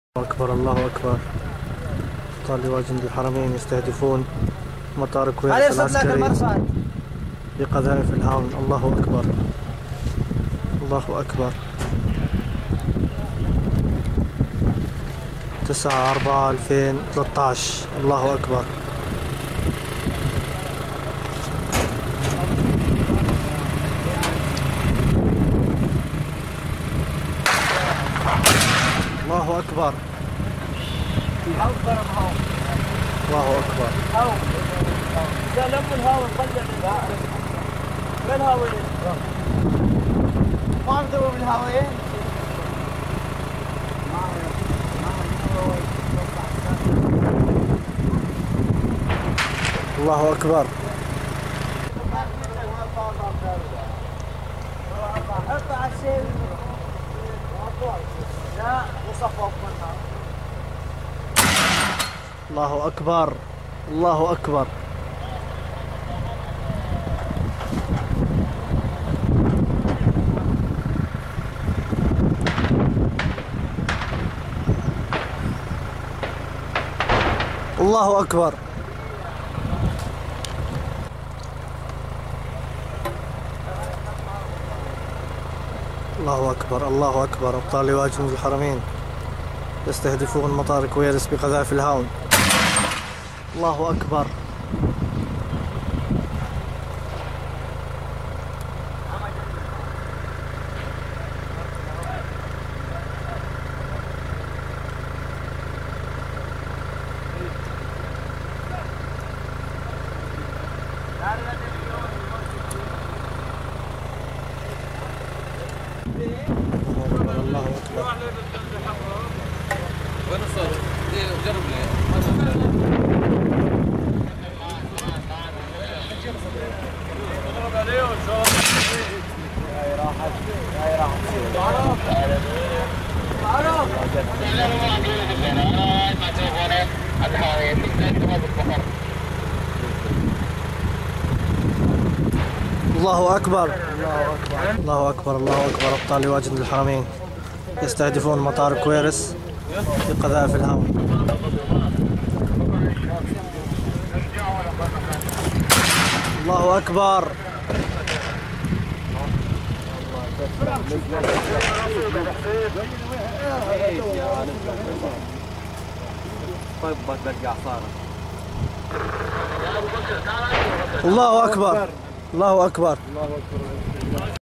شام ريف حلب قصف مطار كويرس العسكري على يد الجيش الحر 9 4 2013
- خاص لشبكة شام الإخبارية